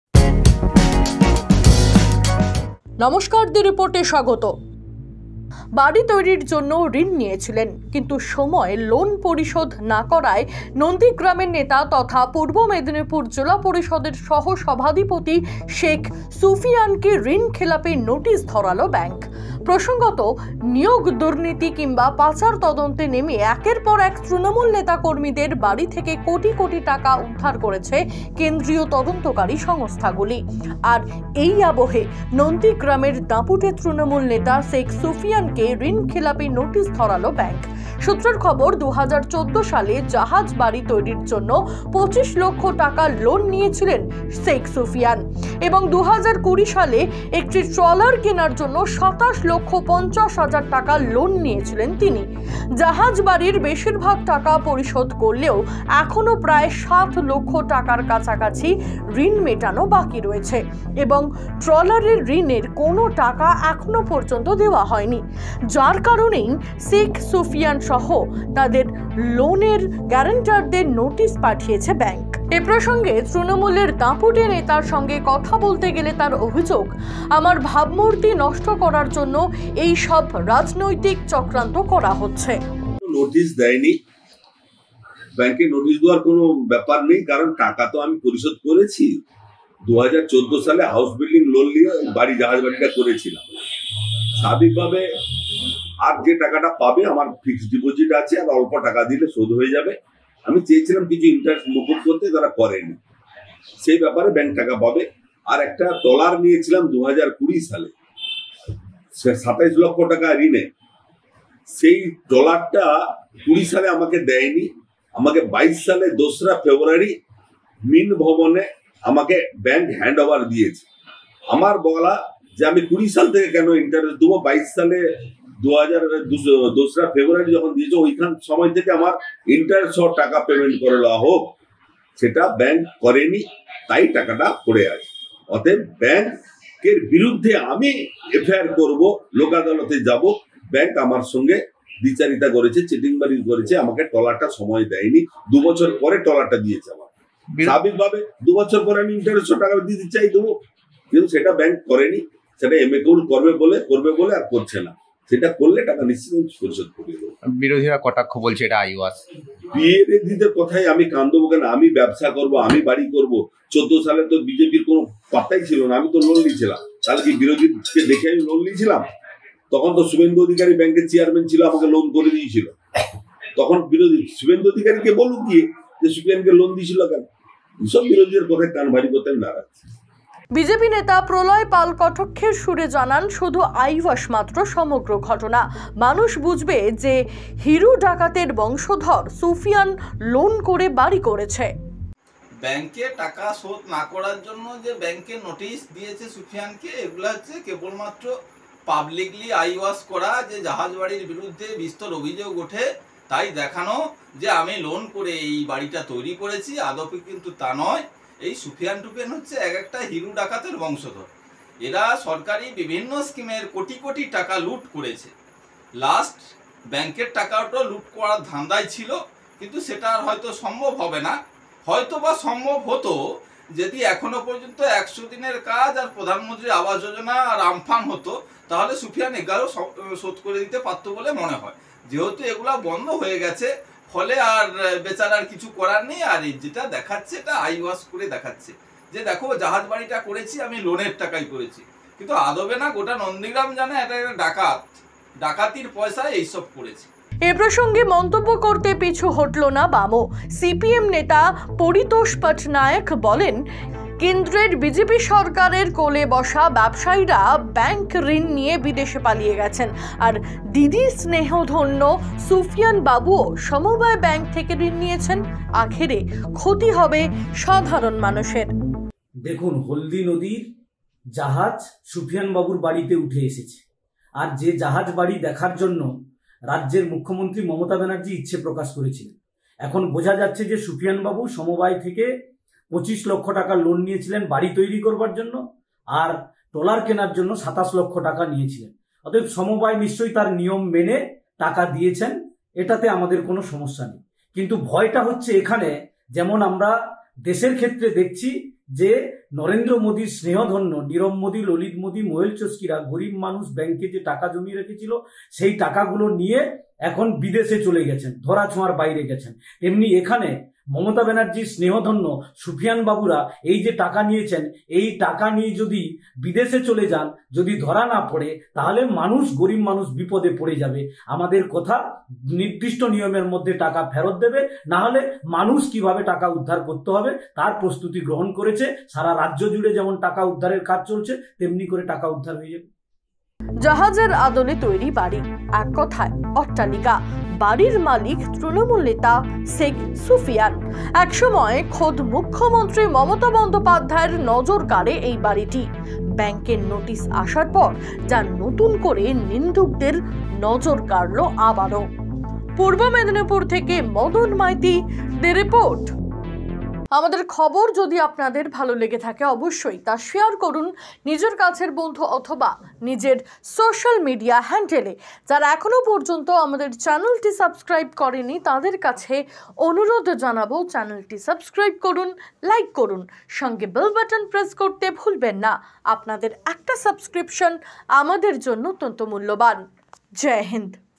East Medinipur Incident:পূর্ব মেদিনীপুর জেলার তৃণমূল নেতা শেখ সুফিয়ানকে ঋণ খেলাপি নোটিস ধরাল ব্যাঙ্ক
ঋন পরিশোধ না করায় পূর্ব মেদিনীপুর জেলা পরিষদের সহ সভাধিপতি শেখ সুফিয়ানকে ঋণ খেলাপি নোটিস ধরাল ব্যাঙ্ক।